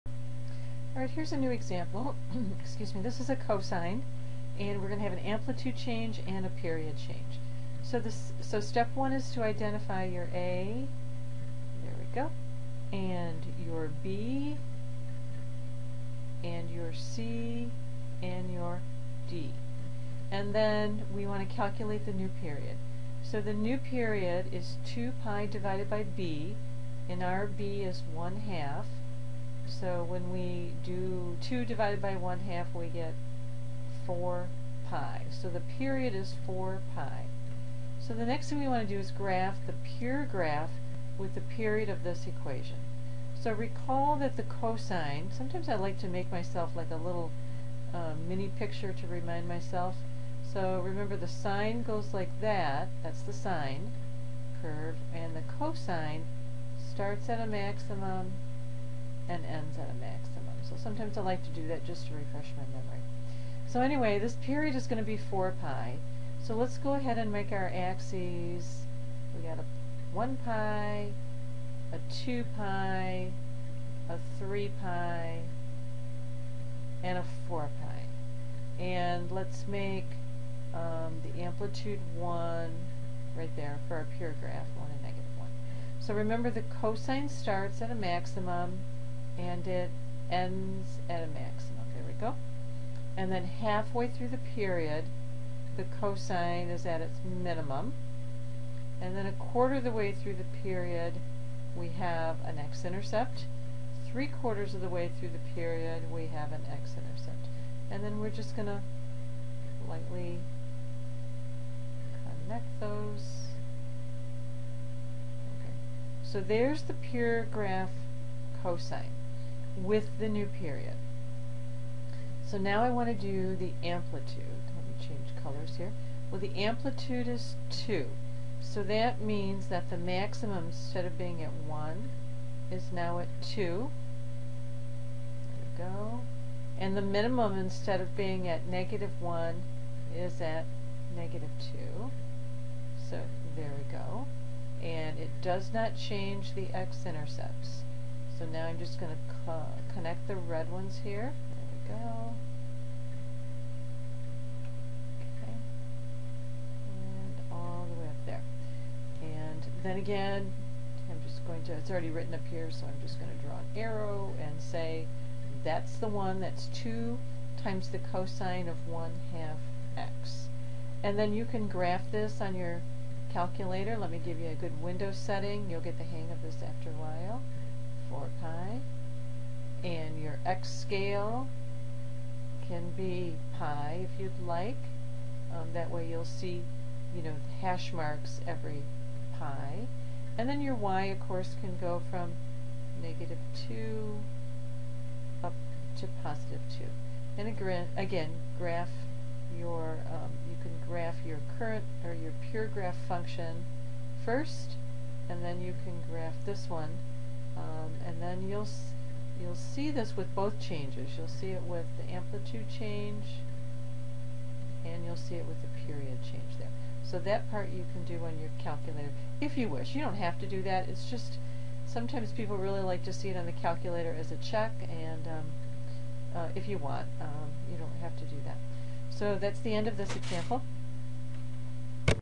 2.1 Graphing Cosine with Amplitude and Period Change
A lesson on graphing the cosine curve that has an amplitude and a period change.